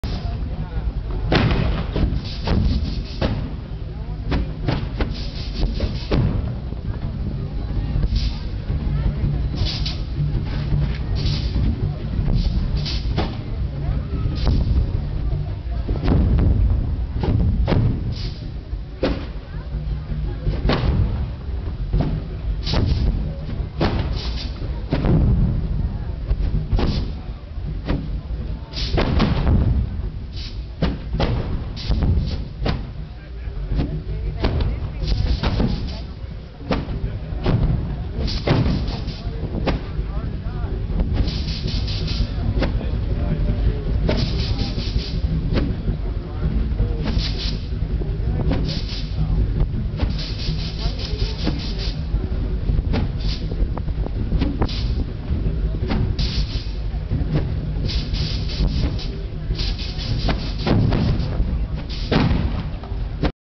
Large Hot Pipe Organ at Robodock
short video of the Large Hot Pipe Organ at Robodock 2007